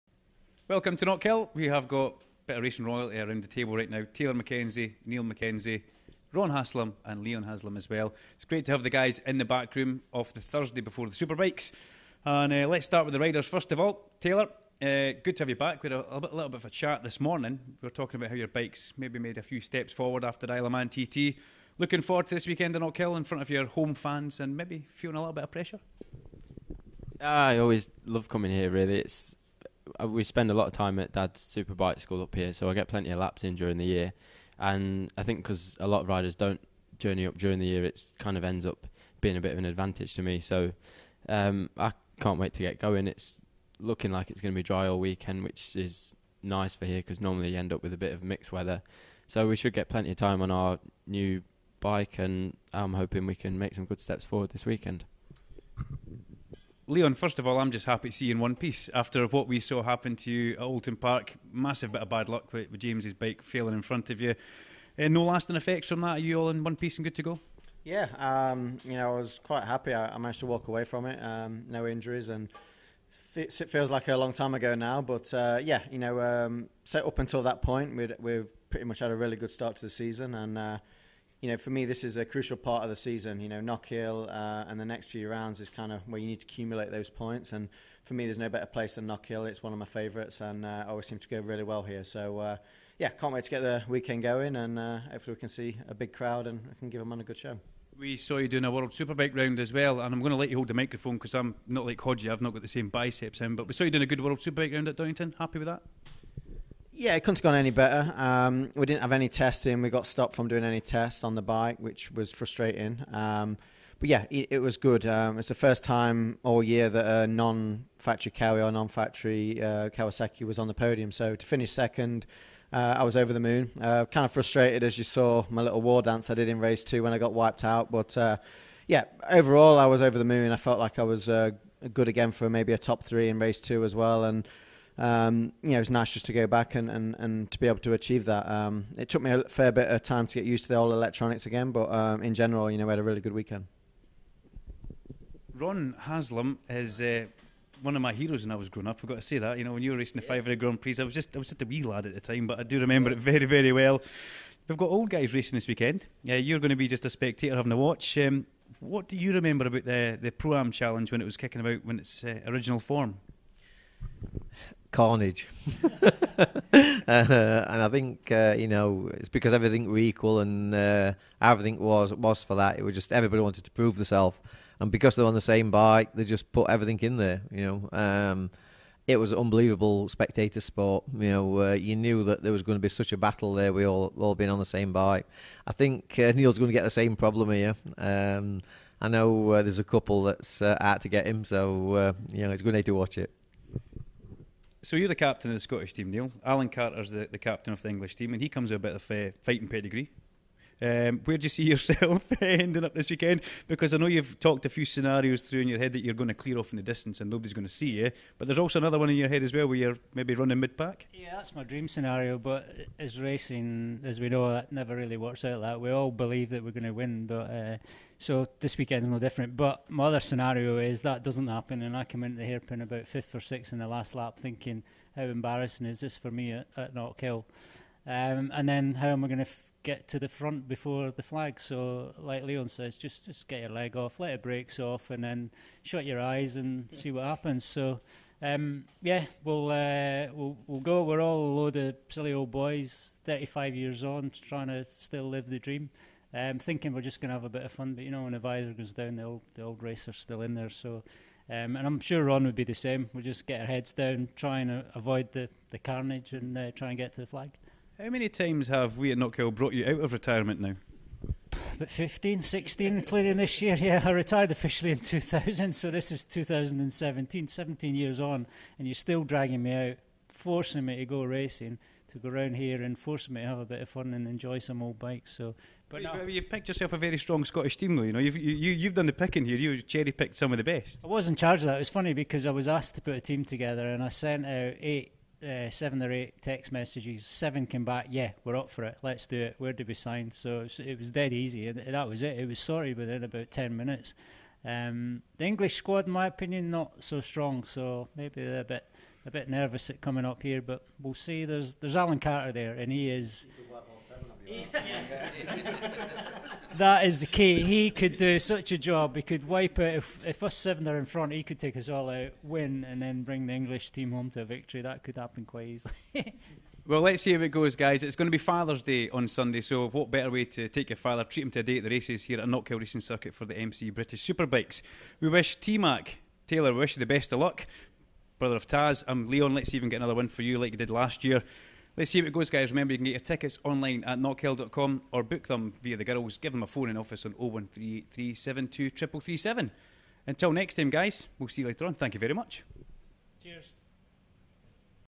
Knockhill BSB with Leon/Ron Haslam and Taylor/Niall Mackenzie
Bit of a pre-BSB catch up with the Haslam's and Mackenzie's. Both Taylor and Leon will be looking for strong results this weekend as the MCE British Superbikes come to Knockhill for round 4 of the series. Whilst, Niall Mackenzie comes out of retirement (again) to race in the Scotland Vs England Pro-Am Challenge that is also taking place at Knockhill this weekend.